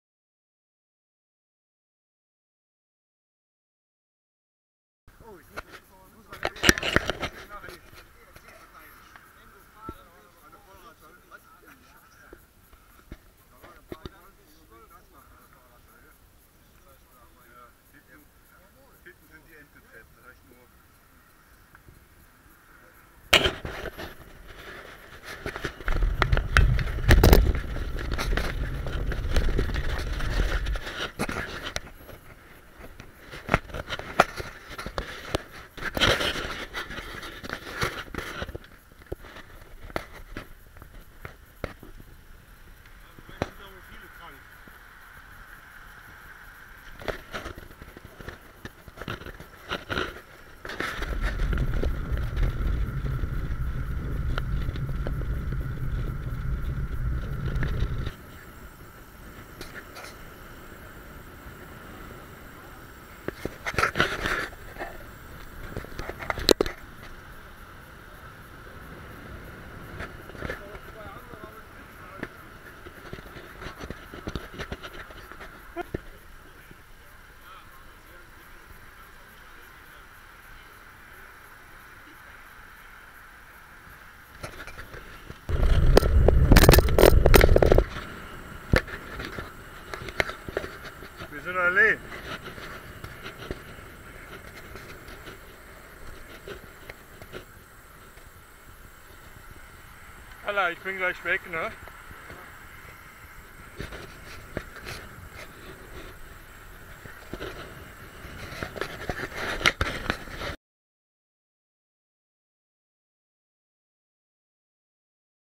Samstagsrunde
Eine Runde mit dem Rennrad und vor dem Spiel wieder zu Hause